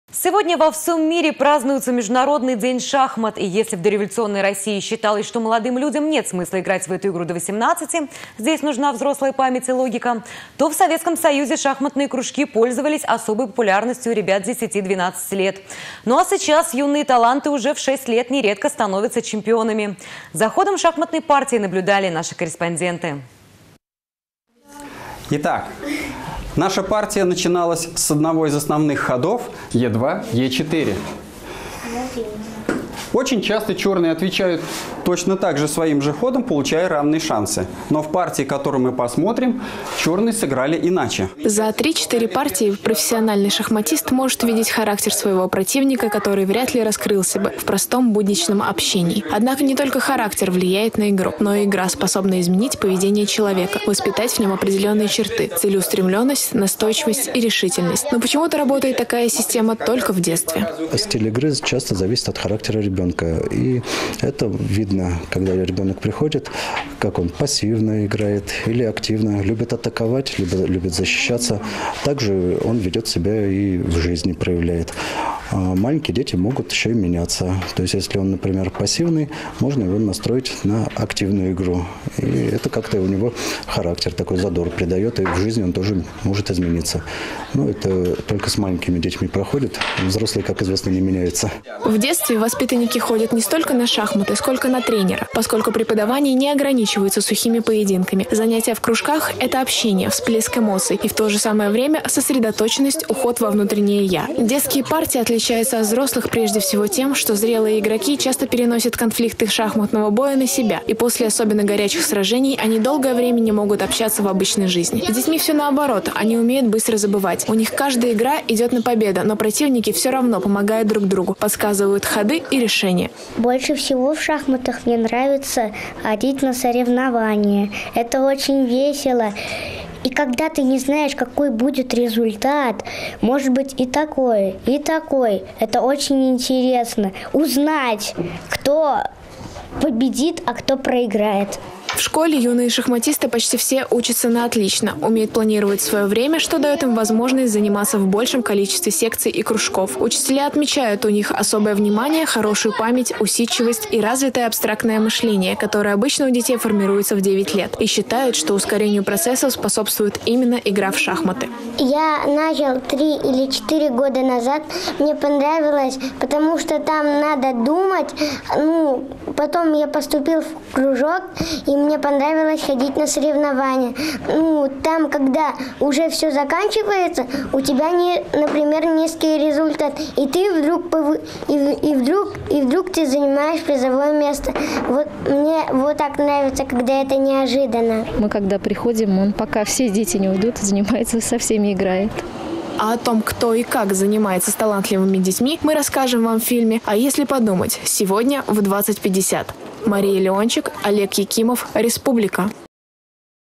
В Чебоксарах отметили Международный день шахмат
Если в дореволюционной России считалось, что молодым людям нет смысла играть в эту игру до 18 лет — здесь нужна взрослая память и логика, то в Советском Союзе  шахматные кружки пользовались особой популярностью у ребят десяти-12 лет. Ну а сейчас юные таланты уже в 6 лет нередко становятся чемпионами. За ходом шахматной партии наблюдали наши корреспонденты.